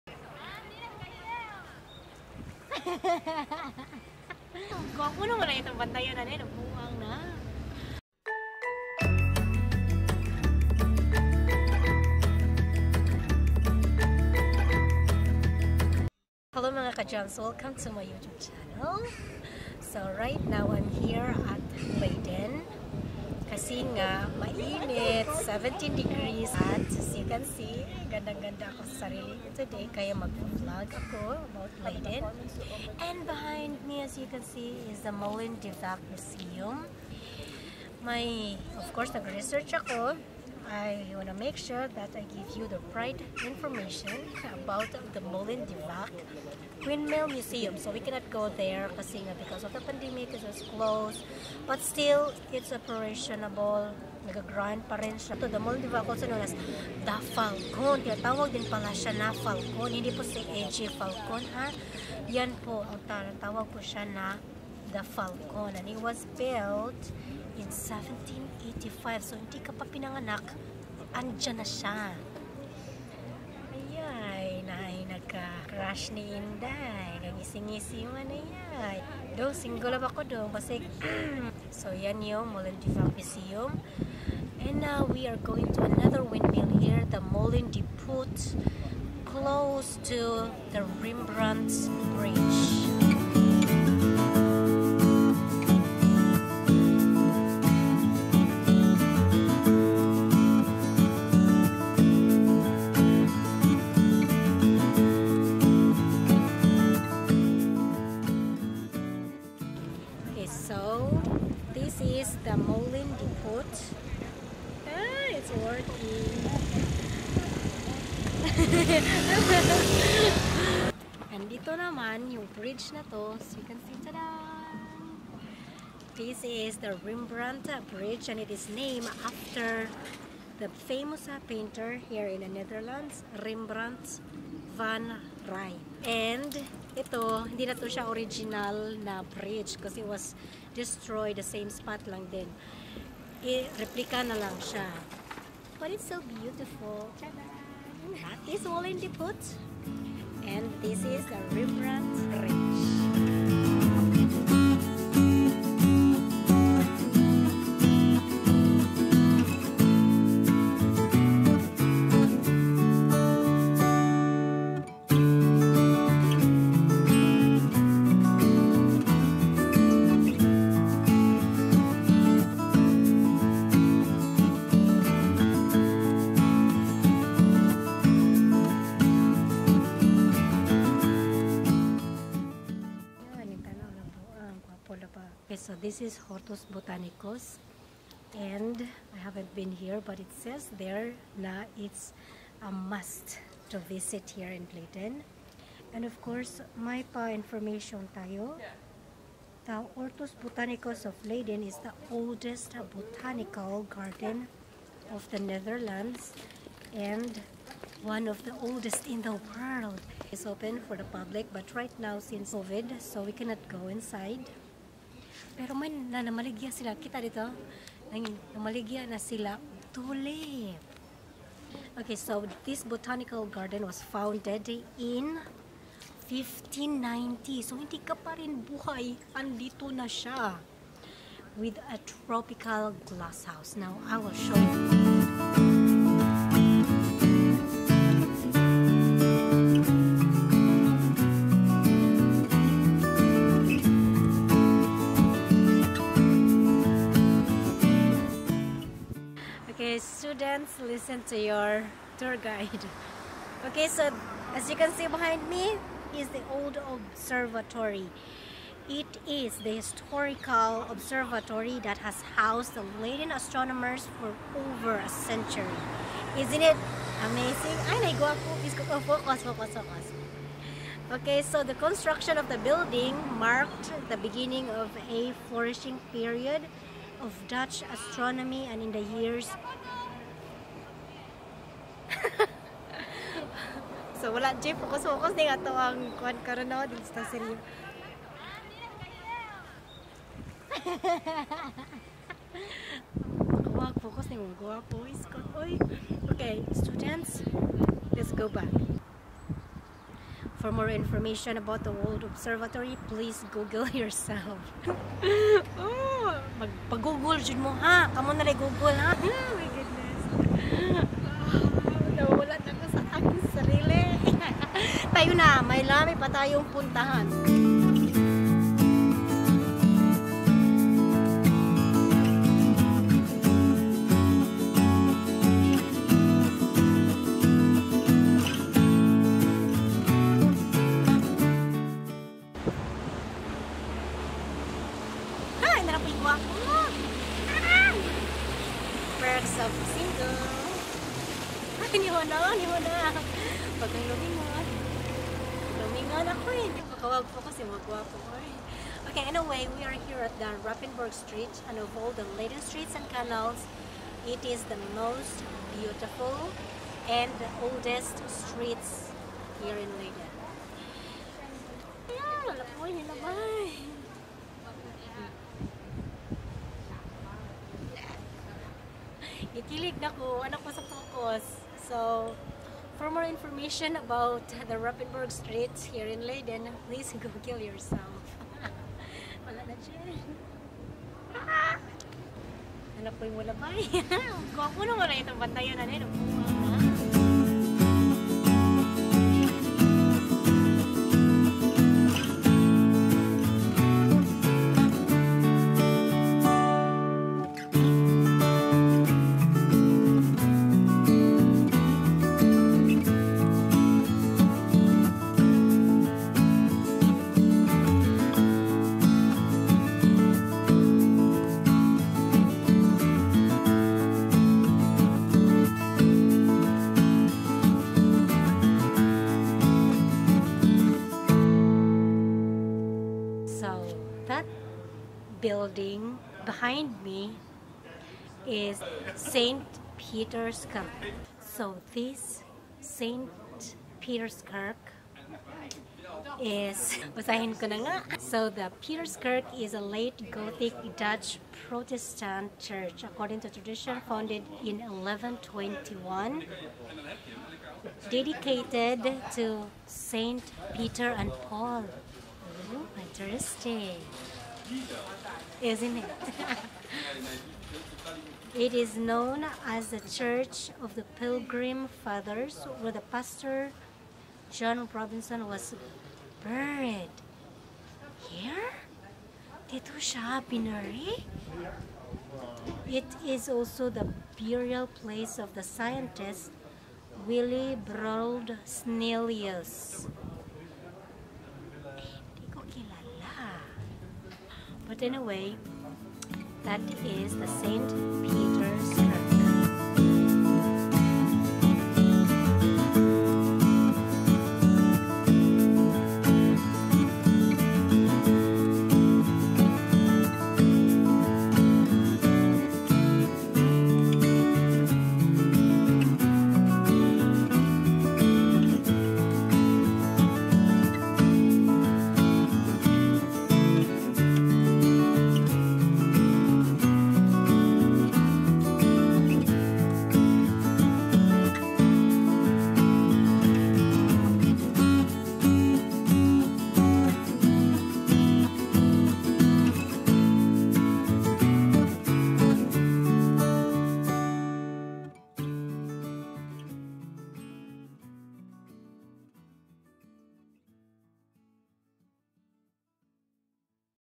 VLOG 3: PLACES TO VISIT IN LEIDEN, THE NETHERLANDS
This is one of what i look forward to visit and experience in the Netherlands and being an aupair is n advantage to explore and learn new places, tradition, history and meet new people. Dutch are nice people.